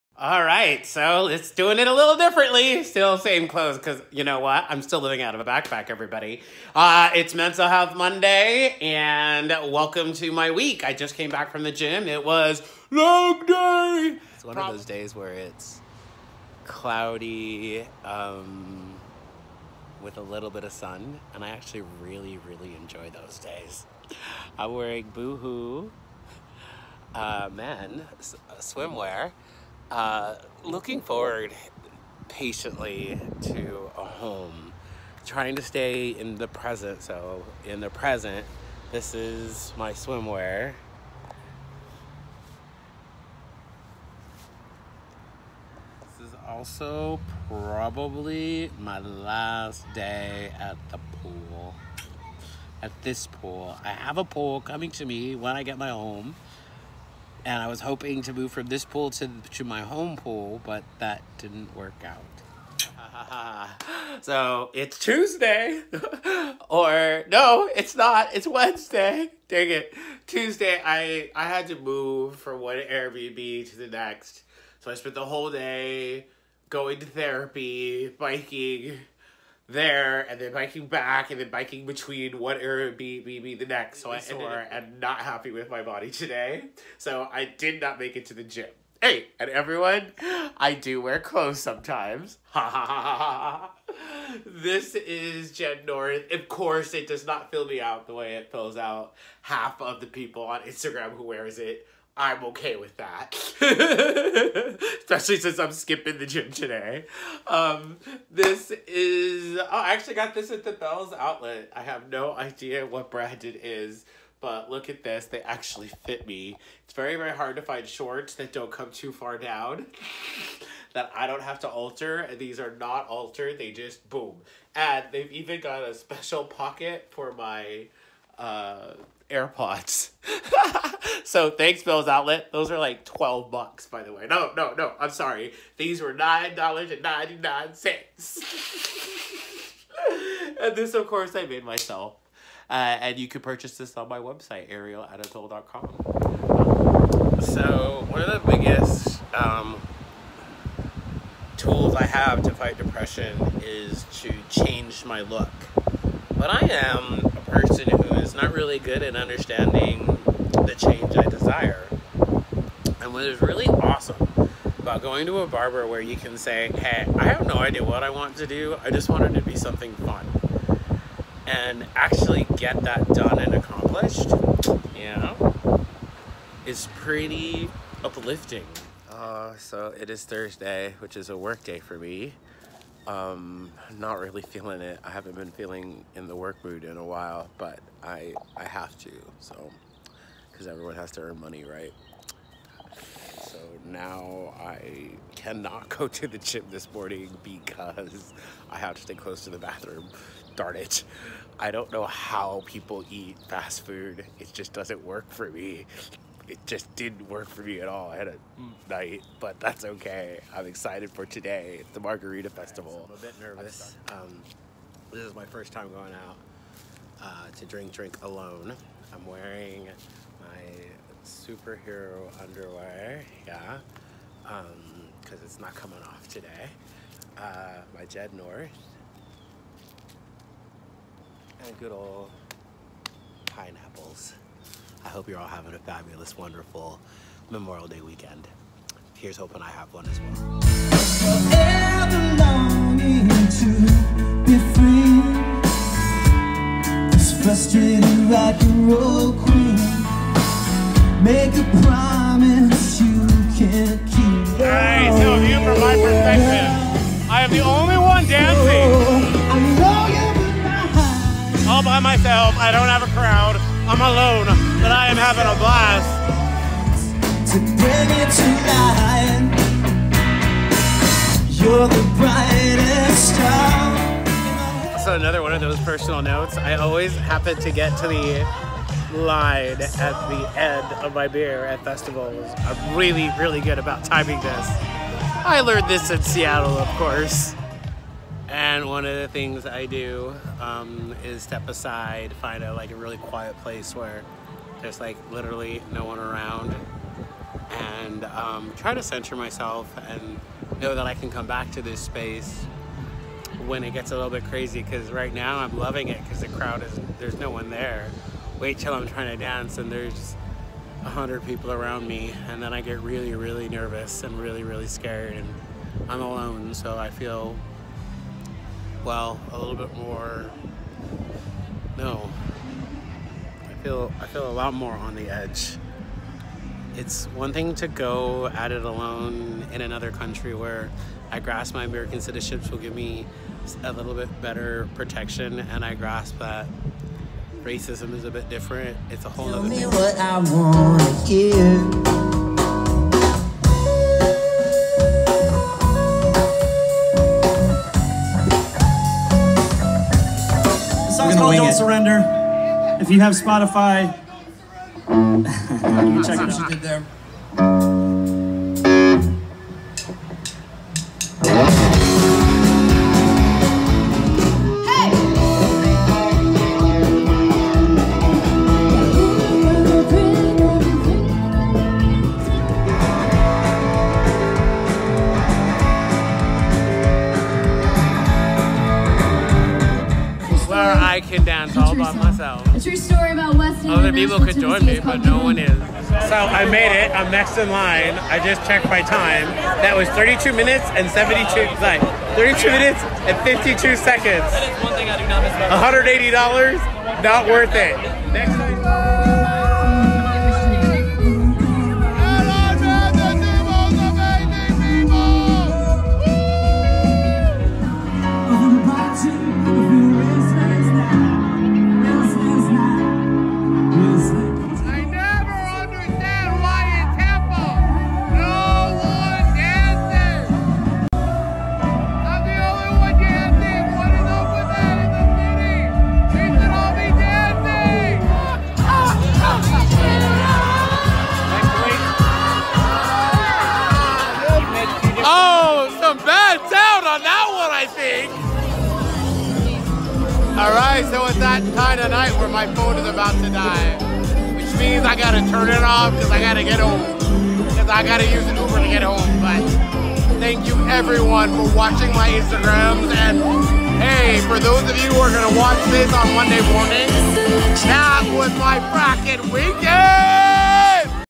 A Crazy Week
Trying what I call 'A Day In The Life' series. A glimpse into how I get through my life. This video is optimized for IGTV feel free to check out my Instagram Page ArielAnatole